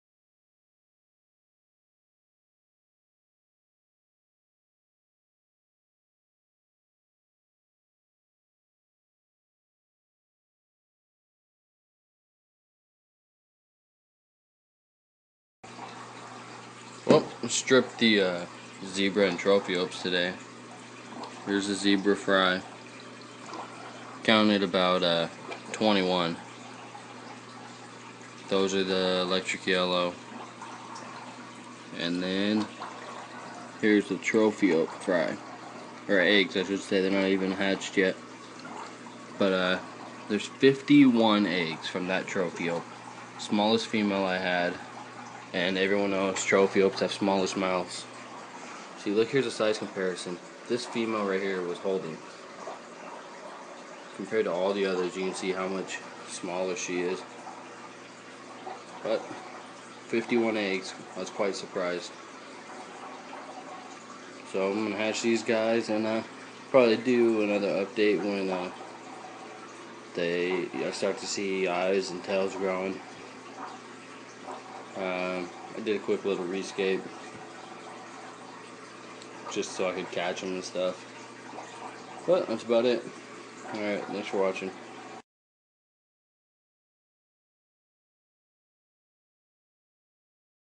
Results From Stripping Holding Females
I stripped my Wild Tropheop Macrophthalmus Chitimba and my Metriaclima Zebra Chilumba Luwino Reef. The troph had 51 eggs and the zebra had 21.